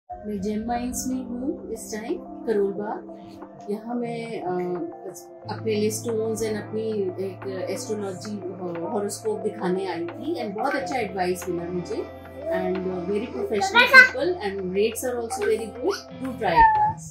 Happy and satisfied client of Gem Mines | Pooja Nagpal
Pooja Nagpal, one of our satisfied customers, came for free horoscope reading from our esteemed and prestigious astrologers.

Certified Gems (by Govt. Lab)
Free Horoscope & Free Consultancy.
Free Testing of Gems & Gold.

Gem Mines
2547 Gali No 6
Beadonpura, Gurudwara road, Karol Bagh, Delhi